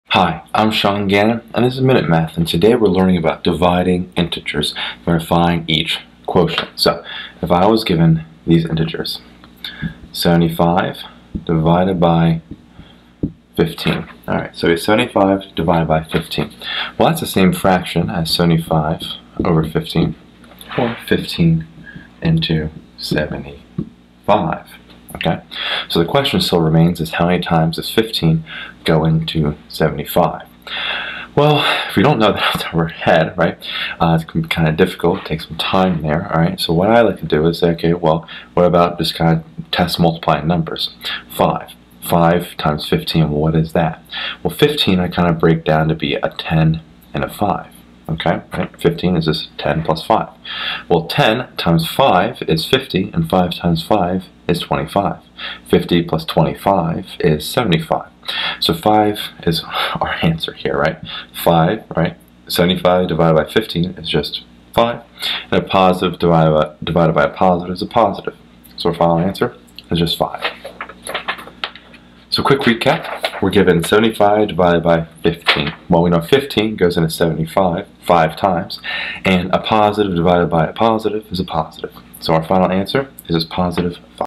Find the Quotient 75÷15
A math video lesson on Dividing Integers. This video finds the quotient of 75/15